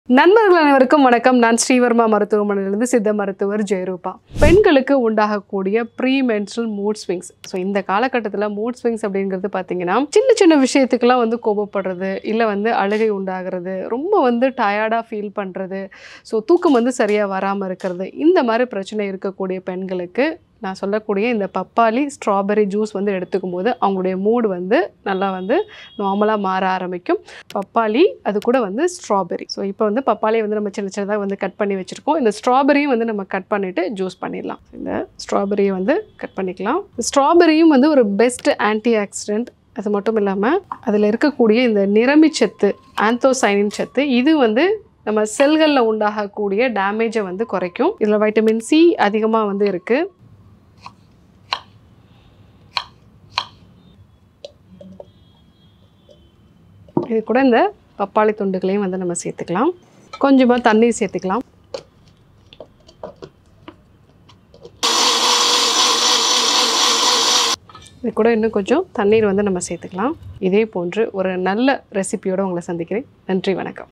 பெண்களுக்கு நன்மை தரும் ஜூஸ் | பெண்களுக்கு மன அழுத்தத்தை போக்கும் மருத்துவ குறிப்பு | Dr.Jeya Roopa
மூட் ஸ்விங்க்ஸ் ஏற்படுத்திடும் மனமாற்றத்தினால் நித்தம் சிரமப்படுகிறீர்களா ? பெண்களுக்கு அடிக்கடி ஏற்படும் கோபம், பதட்டம், உடல் மற்றும் மனச்சோர்வு, தூக்கமின்மை போன்ற பிரச்சனைகளுக்கு பப்பாளி, ஸ்ட்ராபெர்ரி சேர்த்து செய்த "பப்பாளி ஜூஸ்" சிறந்த பானமாக நிவாரணம் அளிக்கின்றது.

மாதவிடாய் வரும் சமயம் ஈஸ்ட்ரோஜன், புரோஜெஸ்ட்டிரோன் ஹார்மோன்ஸ் அளவு குறைவதன் காரணமாக மனமாற்றம் ஏற்படும். ஸ்ட்ராபெர்ரி செல்களில் ஏற்படும் பாதிப்பை சரிசெய்கிறது, மேலும் பப்பாளியில் வைட்டமின் A மற்றும் E சத்துக்கள் மற்றும் அதிக ஆன்டிஆக்ஸிடன்ட்ஸ் இருப்பதால் மூட் ஸ்விங்க்ஸ் ஏற்படுத்திடும் பாதிப்புகளில் இருந்து எளிதில் நிவாரணம் பெறமுடியும்.

Dr. Jeya roopa, B.S.M.S, M.D
Medical director
Shree varma ayurveda hospitals

Embark on a holistic odyssey with shreevarma ayurveda!
Your path to wellness begins here.